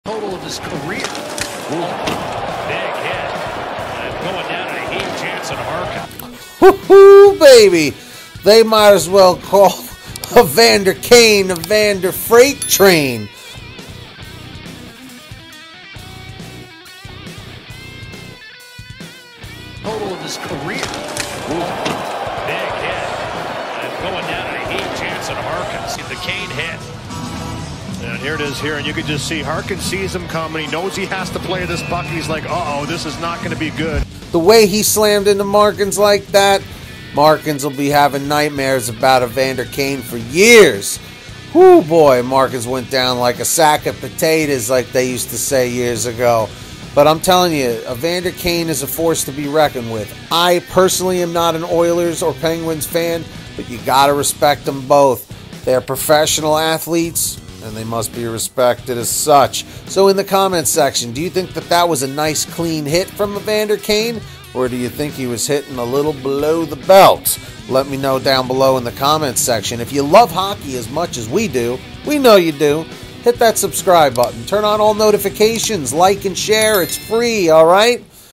Evander Kane HARD Hit on Jansen Harkins (FULL CLIP) Penguins vs Oilers | NHL Highlights
Evander Kane HARD Hit on Jansen Harkins (FULL CLIP) Penguins vs Oilers | NHL Highlights. HA! I caught you reading this! Leave a comment saying "POWER PLAY HEY!" so I know you saw this!